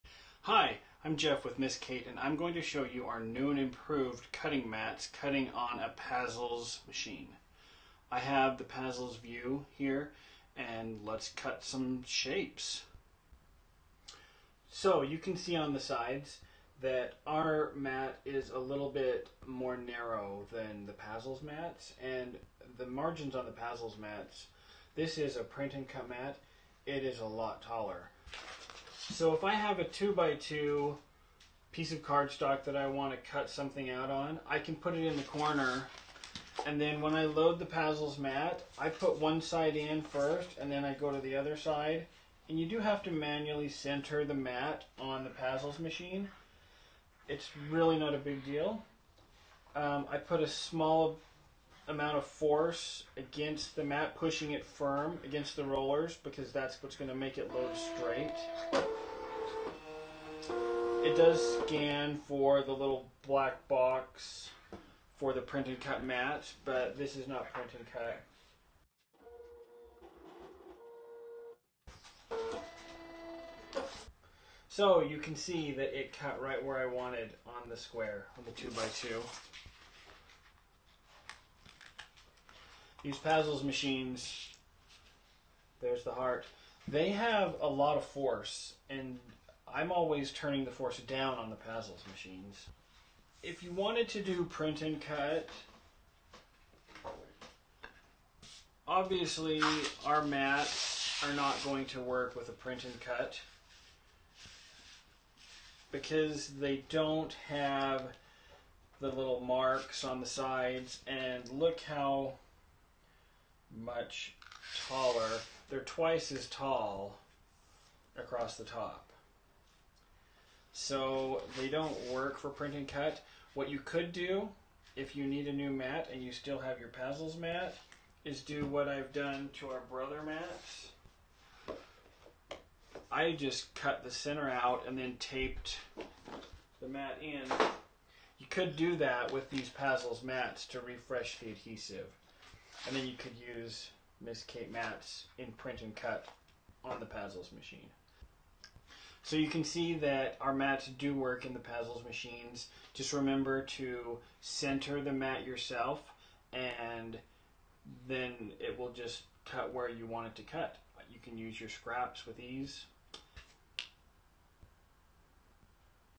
Using Miss Kate Cuttables Cutting Mats with Pazzles Inspiration Vue and InVue
Using Miss Kate Mats with Pazzles Inspiration Vue and InVue

If you manually center the mat in the machine it works great for regular cutting. If you want to use Print & Cut, you will need to modify and combine our mat with a Pazzles Pring and Cut brand mat. Just like I did with the Brother Scan n Cut Mats, (*Credit to Ken Hess of Kens Kreations amazingguy).

Find our Miss Kate Cuttables Supplies

• BBH2.0 (For all Cameo machines EXCEPT 4)